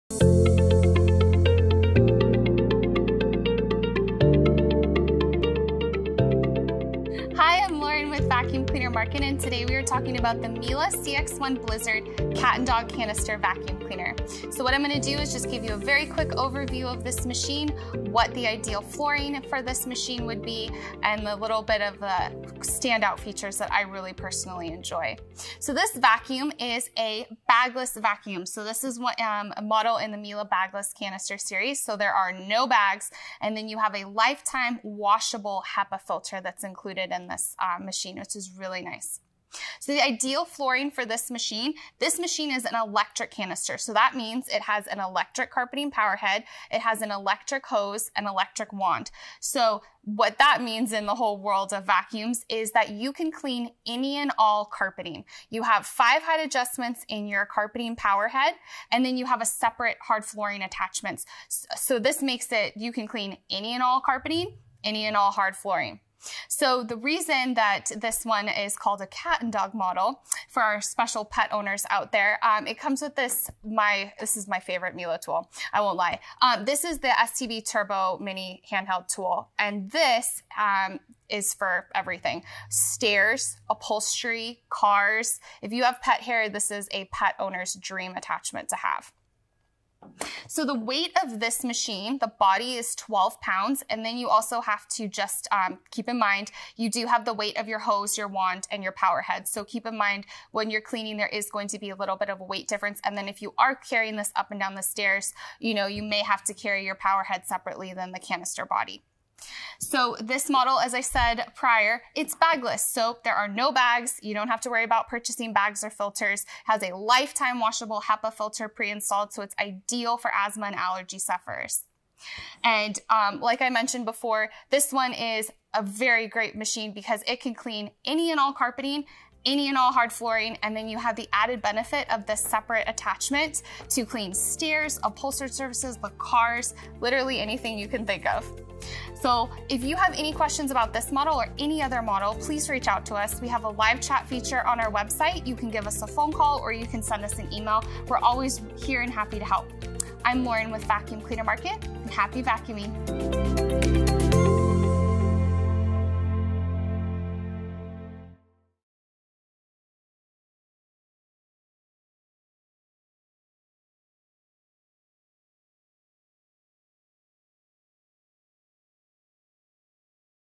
Is This The Right Vacuum For You? Miele Blizzard CX1 Cat & Dog Bagless | VacuumCleanerMarket.com
This machine is a BAGLESS Cat and Dog model, and that means this machine comes standard with a lifetime washable HEPA filter. Completely sealed, this machine is ideal for asthma sufferers and pet owners, or anyone looking for a higher level of filtration. Don't let the name fool you- this machine isn't only for pet owners- it's great for people who hate that "vacuum smell" or live in an area where dust, and debris make their vacuum smell.

This machine has one touch suction controls, 6 suction settings, and integrated tool storage. Your specialty cleaning attachments are stored safely inside their own compartment, so they are never lost, and easy to store.

A separate parquet hard flooring attachment swivels in any direction, and dusts and vacuums your hard flooring simultaneously. The bristles of varied lengths allow you to get deep into cracks and crevices and tile grout on hard flooring. The design of this head turns on a dime and lays flat. Get right up next to your baseboards, around furniture and furniture legs, and whisk up pet hair. This tool was designed with pet owners in mind, so you never need to sweep again! Simply vacuum and mop your flooring. What a time saver.

This machine is a dream on pet hair- as it's name implies. This model comes with Miele's STB 101 turbo handheld mini turbo attachment. This attachment can connect to the base of your handle, or the end of your wand. Clean your cat towers, pet beds, mattresses, and this is our recommended attachment for carpeted stairs. This attachment is also extremely useful in RV's, boats, and cars!